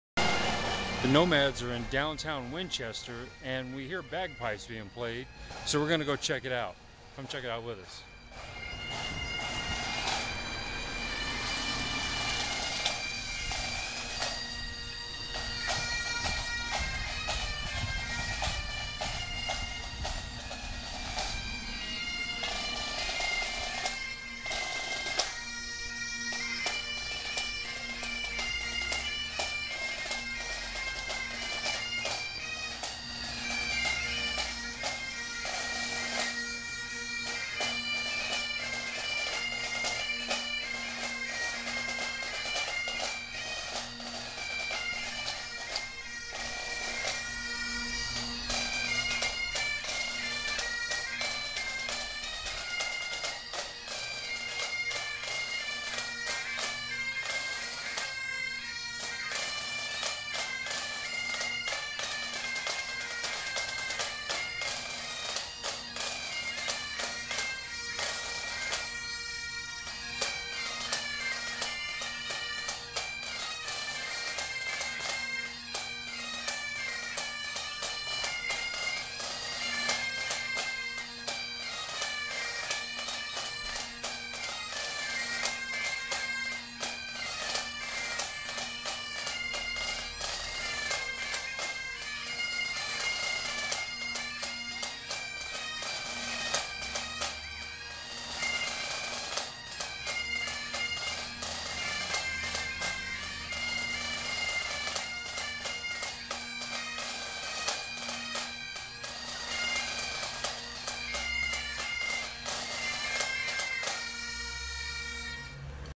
Bag pipes in Winchester
While exploring Winchester, Virginia, we came upon a group of men playing bag pipes. We had to stop and listen for a few minutes.